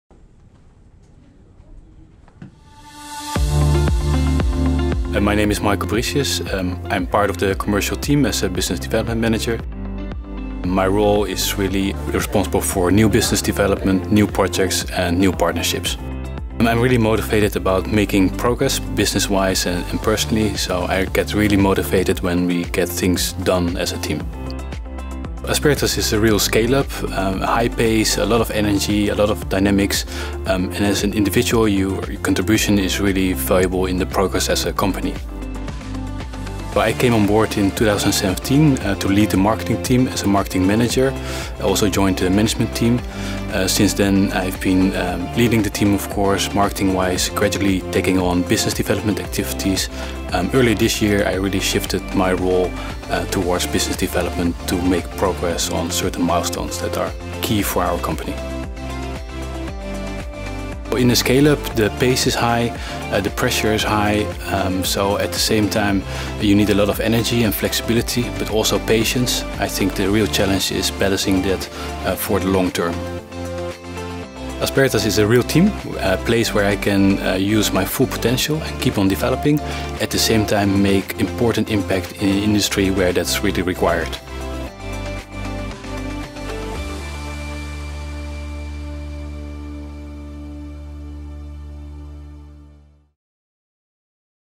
Working at Asperitas – Business Development
Working at Asperitas

Asperitas is a real scale-up – high pace with a lot of energy and dynamics. As an individual, your contribution is really valuable to the progress of the company.

If you:

☑️ share our mission: Cleantech & Sustainability
☑️ have high-tech skills
☑️ are interested in the datacentre industry
☑️ are ready to go with lots of enthusiasm and energy

Then one of our open positions may be for you.